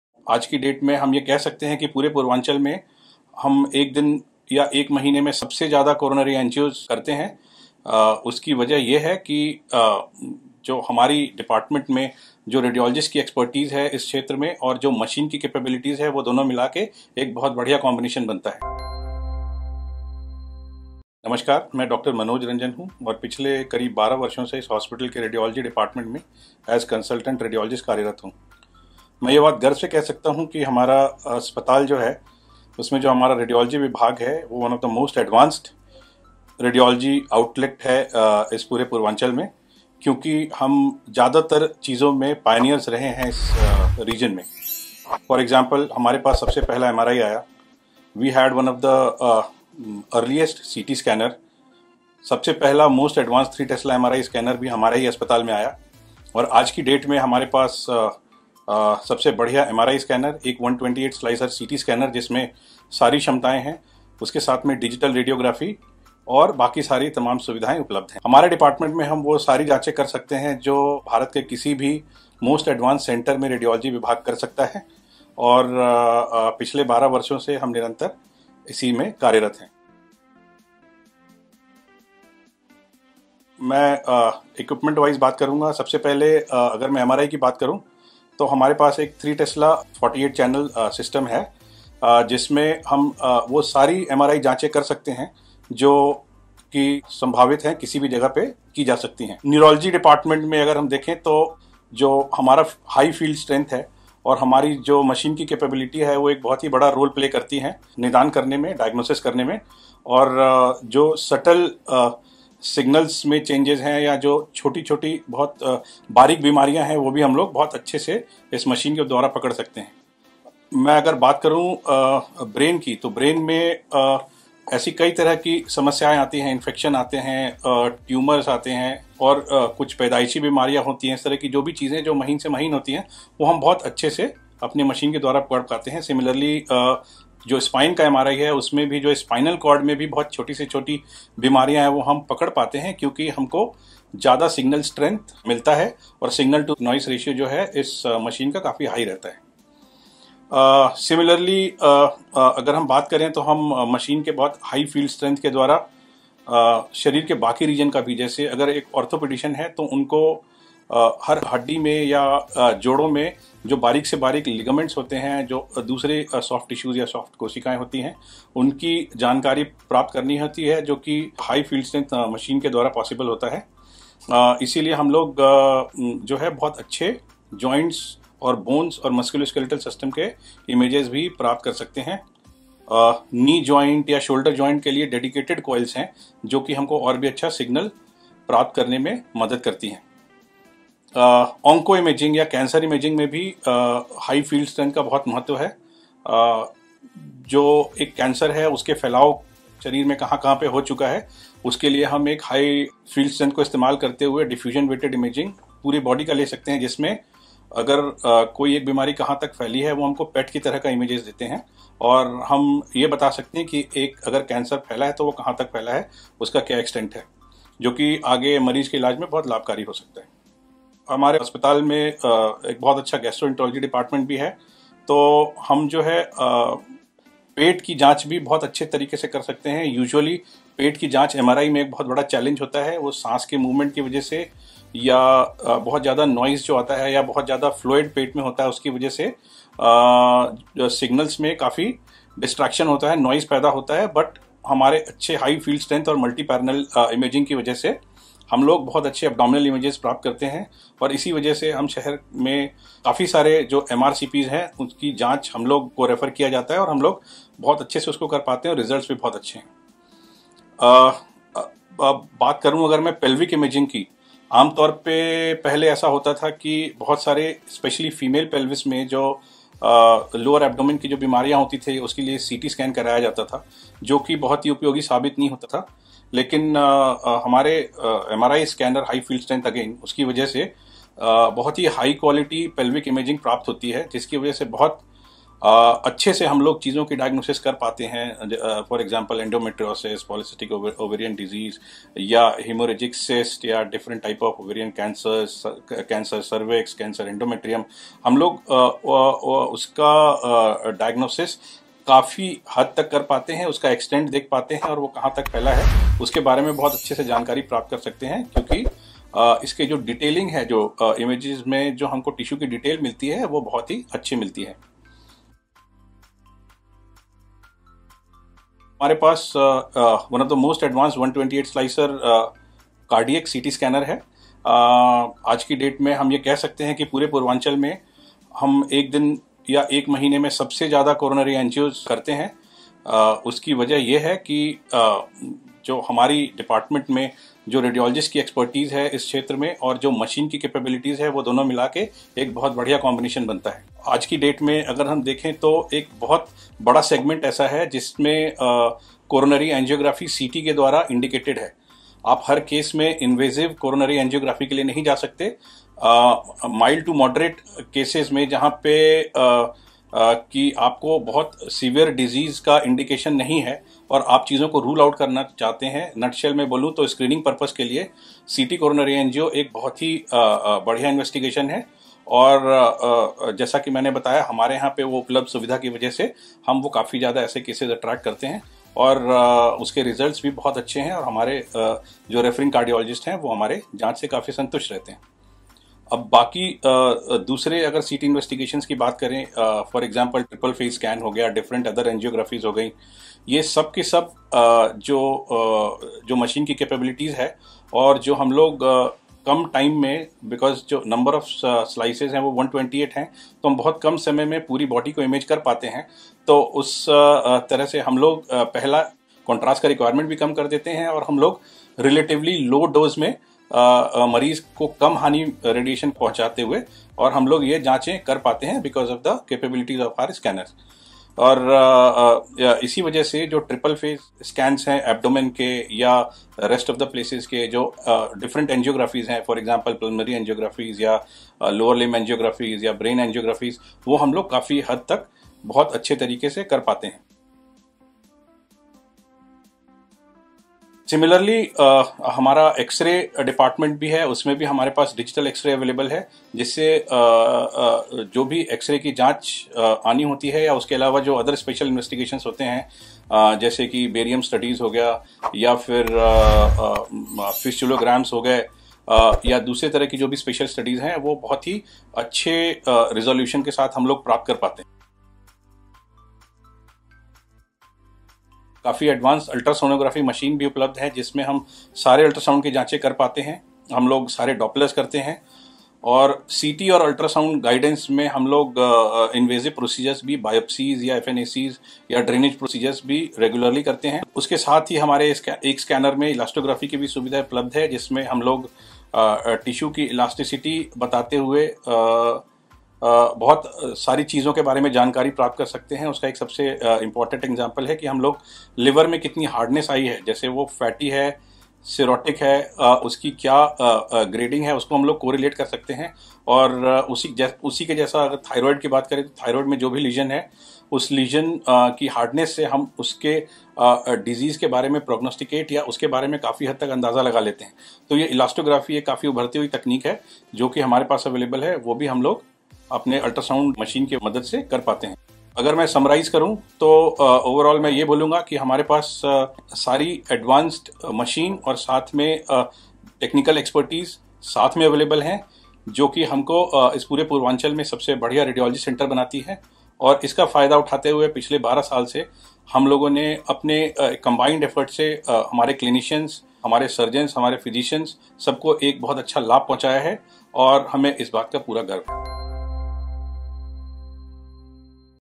Best Diagnostic Center In Purvanchal - Heritage Hospitals
हेरिटेज हॉस्पिटल का रेडियोलॉजी विभाग पूर्वांचल का सबसे उन्नत एवं एडवांस, अत्याधुनिक सुविधाओं से परिपूर्ण डायग्नोस्टिक्स सेण्टर है।

हेरिटेज हॉस्पिटल अत्याधुनिक मशीनों से पिछले कई वर्षो से पूर्वांचल के मरीजों को सटीक परिणाम के साथ बेहतर परामर्श प्रदान करा रहें है।

हेरिटेज हॉस्पिटल के डायग्नोस्टिक सेण्टर में चिकित्सकीय आधुनिकता के क्षेत्र में सबसे अग्रिम रहें हैं, जैसे कि पूर्वांचल की सबसे पहली MRI मशीन, सबसे पहली CT स्कैनर और सबसे पहला 3 टेस्ला एडवांस्ड MRI स्कैनर, हेरिटेज हॉस्पिटल को सबसे बेहतरीन निदान केंद्र बनाने में सहायक हैं।

डॉ. मनोज रंजन (रेडियोलॉजिस्ट कंसलटेंट) इस विडियो के द्वारा बता रहें हैं कि डायग्नोस्टिक सेण्टर में कौन सी मशीने किस प्रकार से काम करतीं है और किस मशीन का उपयोग किस बीमारी का निदान करने के लिए किया जाता है।